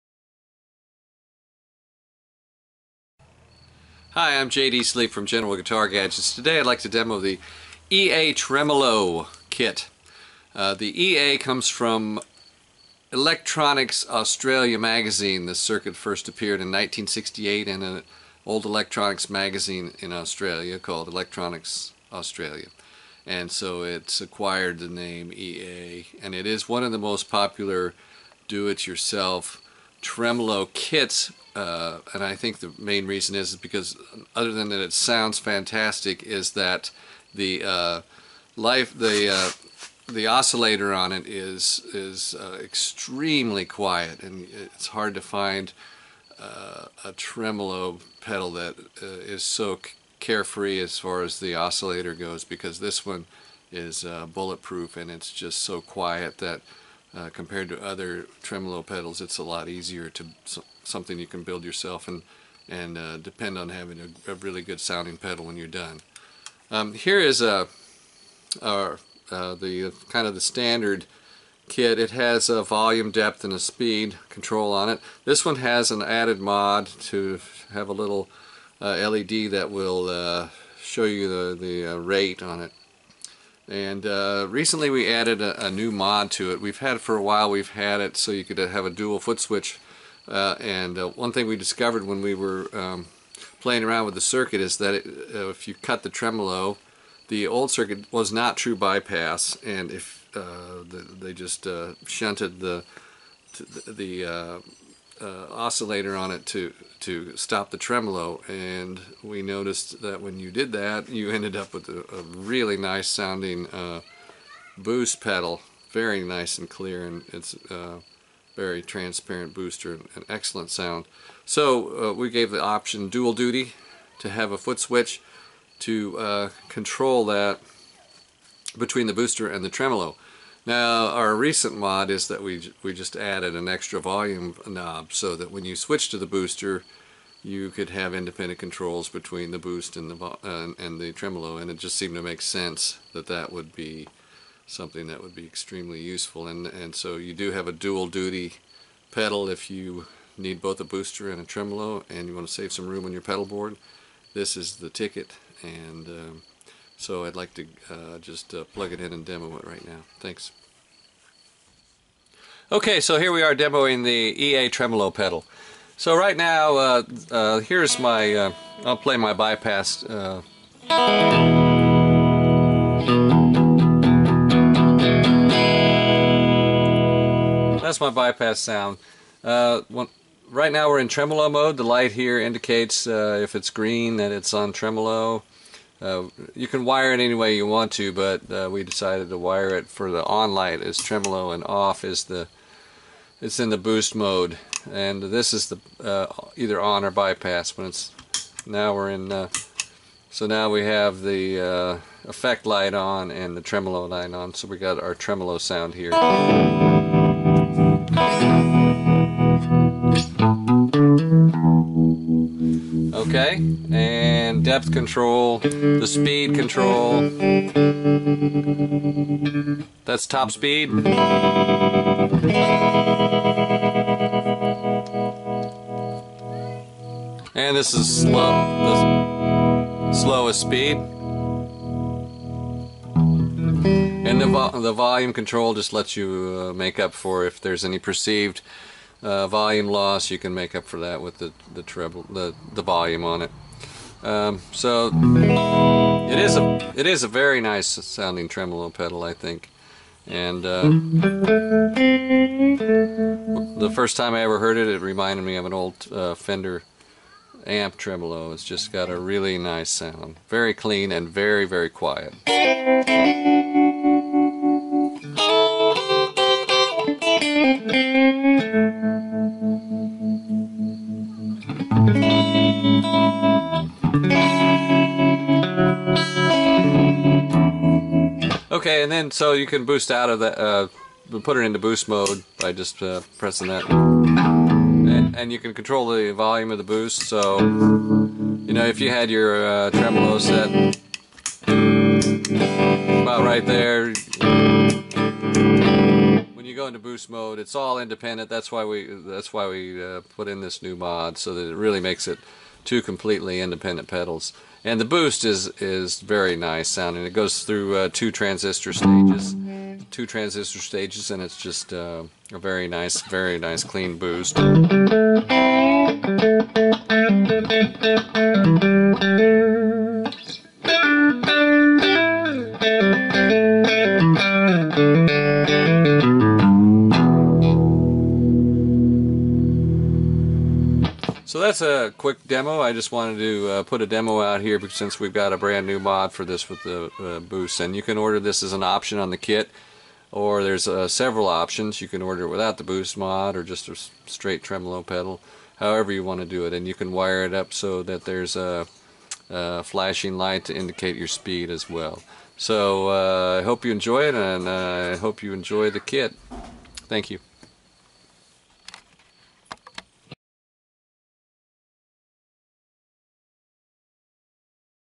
GGG EA Tremolo Demo
Demo of a great modification to the EA Tremolo Kit.  Check it out at generalguitargadgets.com.